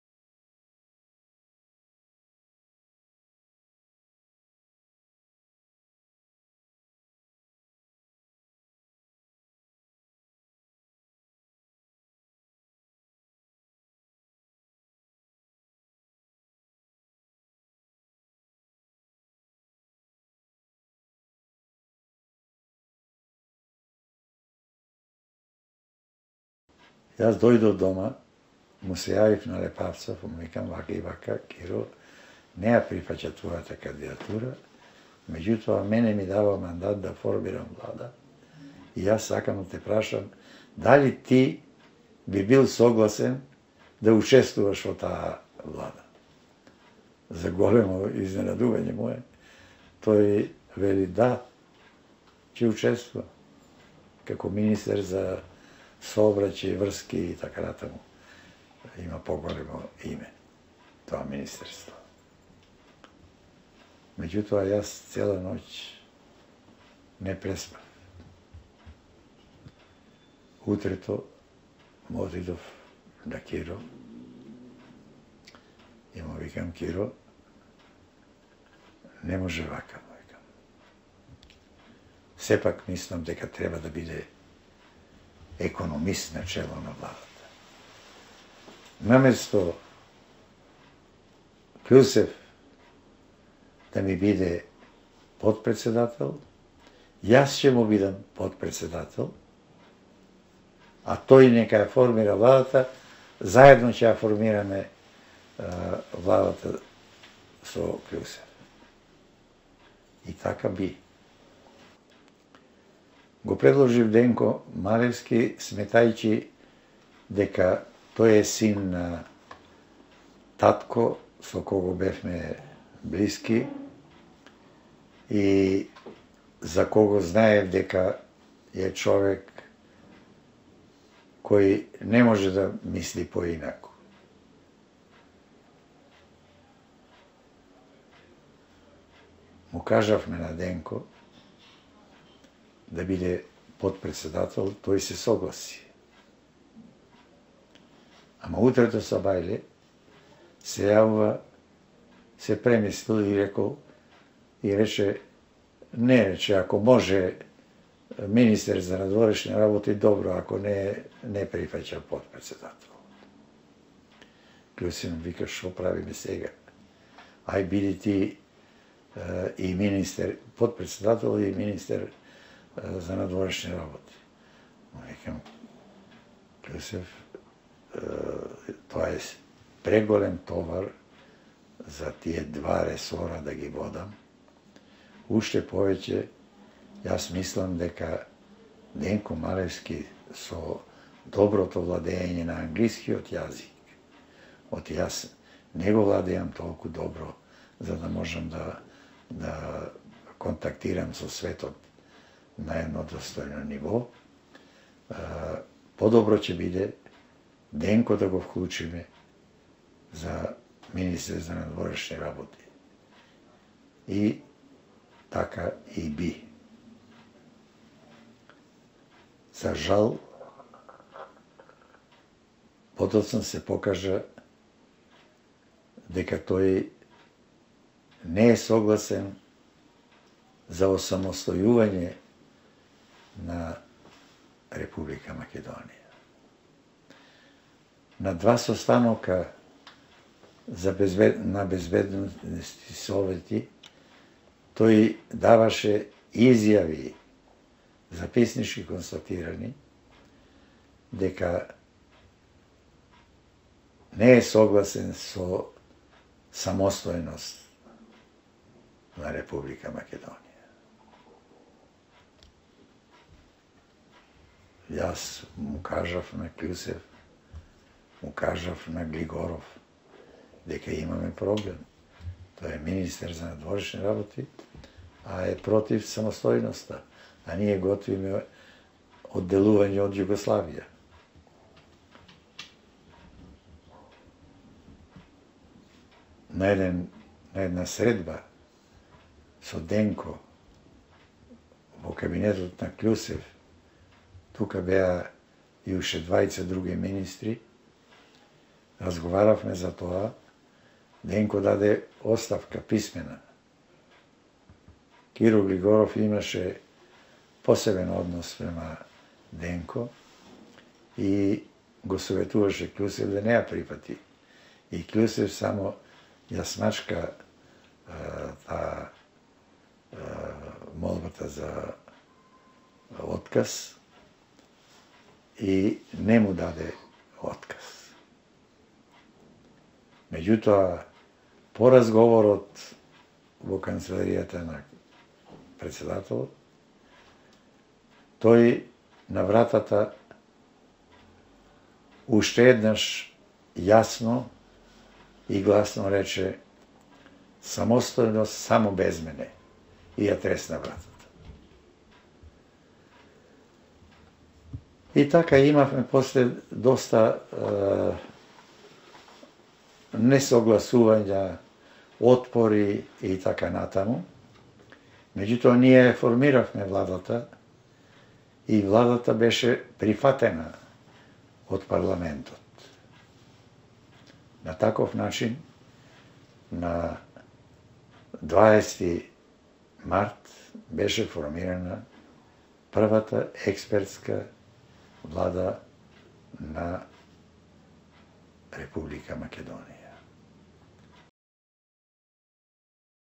Blaze Ristovski about Denko Maleski
Blaze Ristovski (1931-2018) Macedonian historian, folklorist, Academician, member of MANU, Vice Prime Minister testifies on the formation of the first "expert" Government in independent Macedonia in 1991 and how Denko Maleski  didn't want to accept the independence of Macedonia and its separation from Yugoslavia.
(This video, without the English subtitles, was originally published on Facebook by Dime Ratajkoski and is composed of fragments form a new project - contributions for the newer Macedonian history. The video memoirs of Academician Blaze Ristovski are made shortly before his passing.)

Блаже Ристовски (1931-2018) македонски историчар, фолклорист, Академик, член на МАНУ, Вицепремиер сведочи како е формирана првата „експертска“ Влада во независна Македонија во 1991 г. и за тоа како Денко Малески одбивал да ги прифати независноста на Македонија и нејзиното одделување од Југославија.
(Видеото, без англискиот превод, е оригинално објавено на Фејсбук од Диме Ратајкоски и содржи фрагменти од нов проект – прилози кон современата македонска историја. Видео мемоарите со академик Блаже Ристовски се направени непосредно пред да почине.)